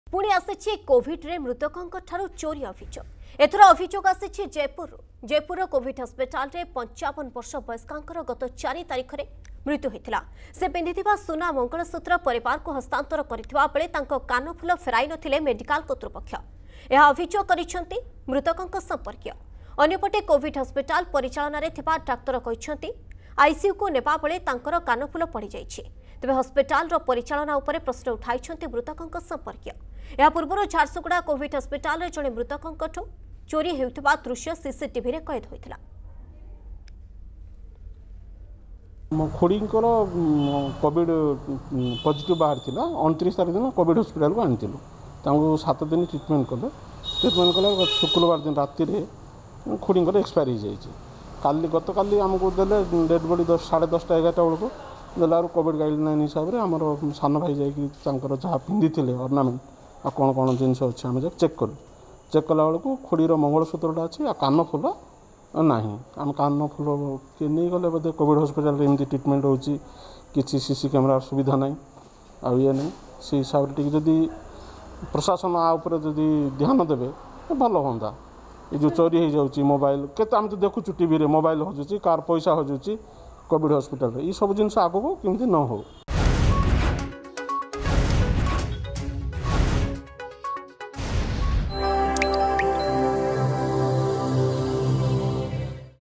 ପୁଣି ଆସିଛି କୋଭିଡରେ ମୃତକଙ୍କଠାରୁ ଚୋରି ଅଭିଯୋଗ
ପୁଣି ଆସିଛି କୋଭିଡରେ ମୃତକଙ୍କଠାରୁ ଚୋରି ଅଭିଯୋଗ। ଏଥର ଅଭିଯୋଗ ଆସିଛି ଜୟପୁରରୁ। ଜୟପୁର କୋଭିଡ ହସ୍ପିଟାଲରେ ୫୫ ବର୍ଷ ବୟସ୍କାଙ୍କର ଗତ ୪ ତାରିଖରେ ମୃତ୍ୟୁ ହୋଇଥିଲା।